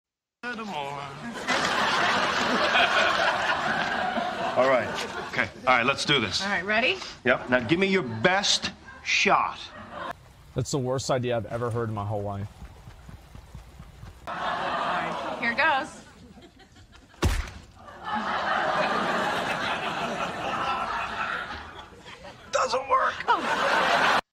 "Worst idea ever," Joey gets kicked in the nuts
A scene where every guy flinches due to its "sensitive" nature.

Asmongold doesn't hold back his thoughts on the situation. What was Joey thinking?